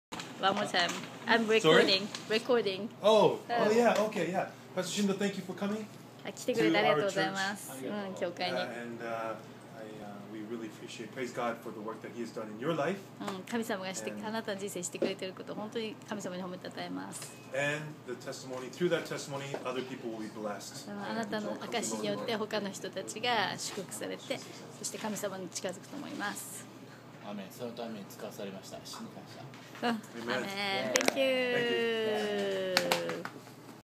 地球村教会チャールズ牧師のグリーティング
カナダ出身のグローバルミニストリー部長